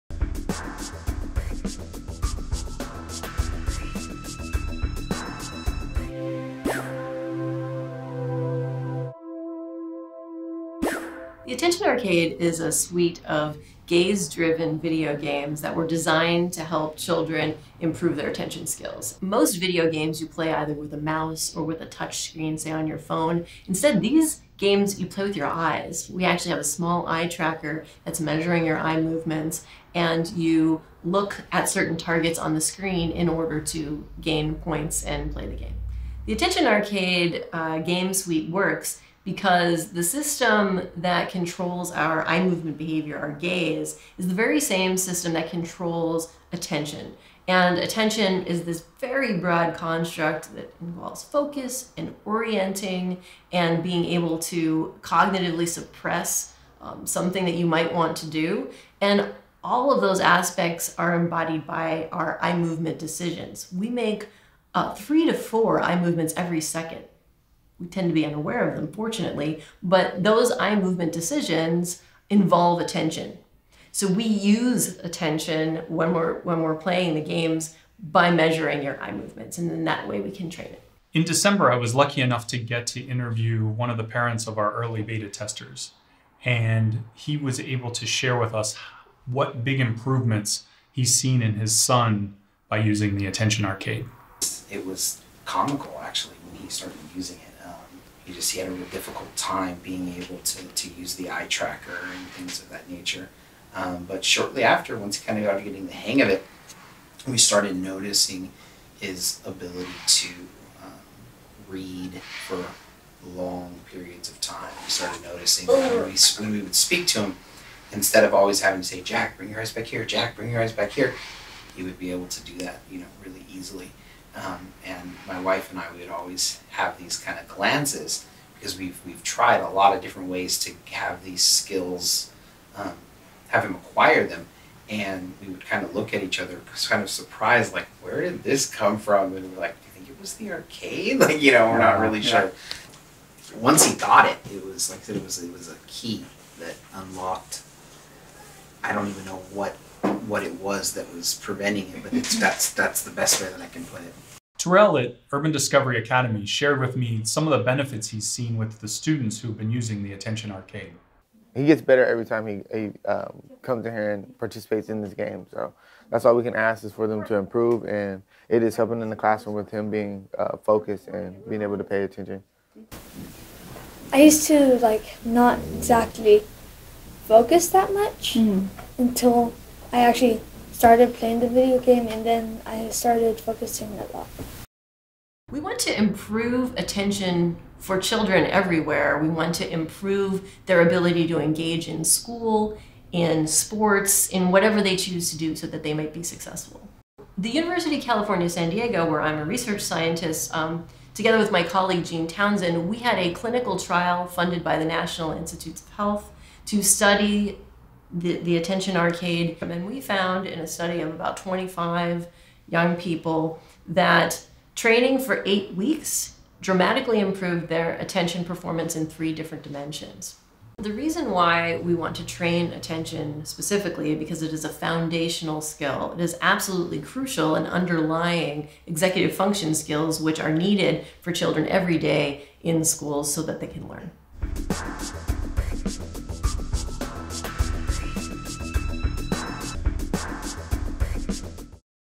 About BrainLeap's Attention Arcade
Learn more about the science behind the games and who it could help
Improving attention in kids doesn't need to be boring and tedious. The Attention Arcade™ is a FUNervention that is research-based. Each of the six attention training games trains a different aspect of attention. Children play the games using an eye tracker, rather than a mouse or keyboard, which increases the intensity of the games and ensures they remained focused on the game - looking away causes a loss of points or their turn to end. We offer both a school solution and an at-home solution.